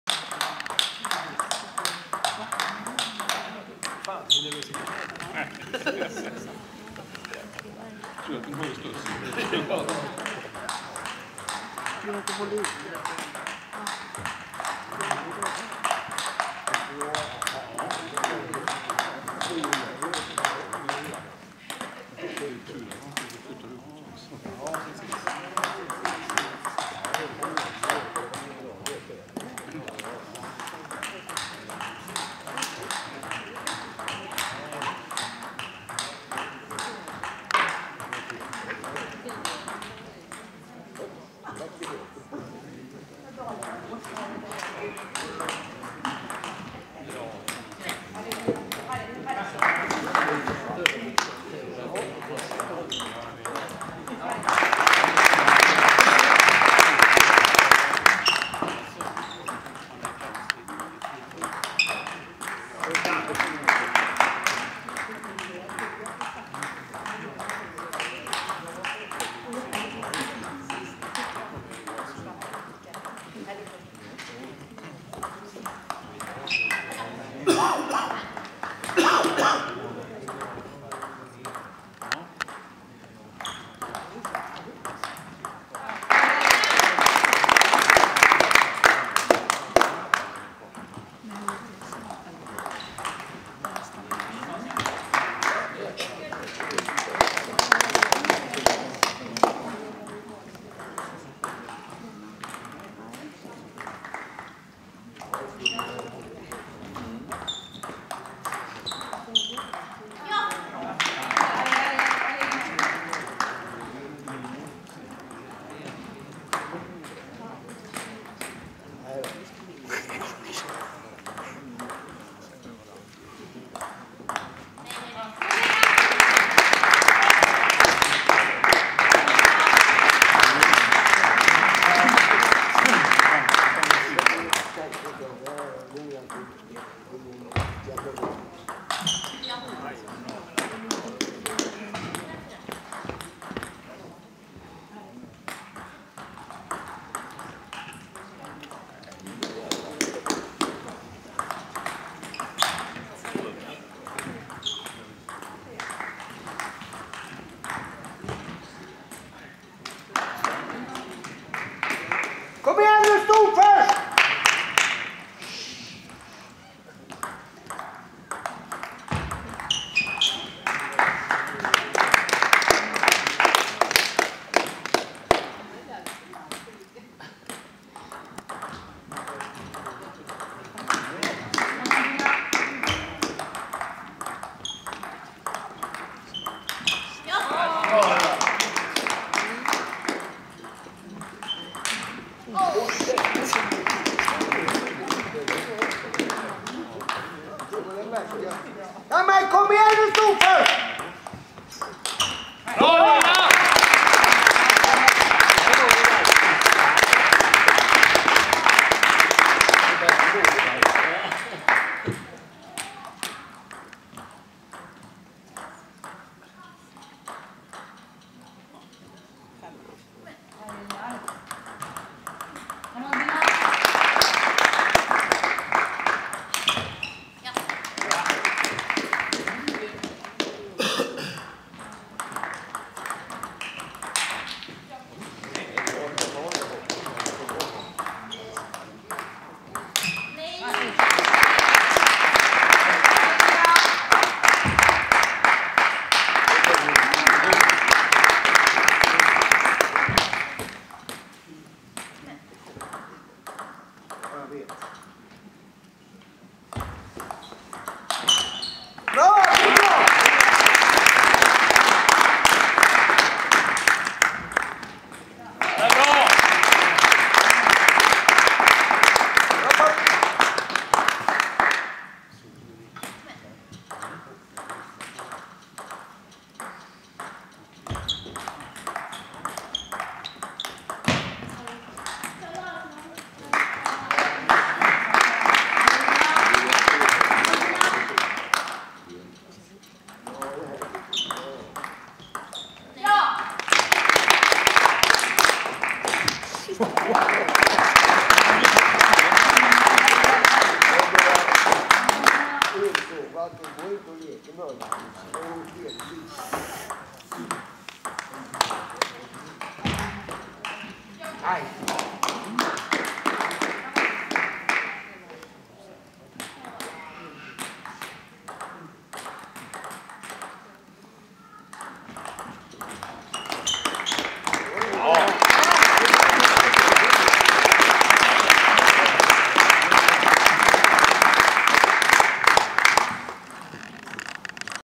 Bordtennismatch Storfors BTK - IK Juno 2018-02-03 (Mickez.se)
Match i Pingisligan för damer i Sporthallen i Storfors. Match mellan Storfors BTK och gästande IK Juno en match Storfors vann med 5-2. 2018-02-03.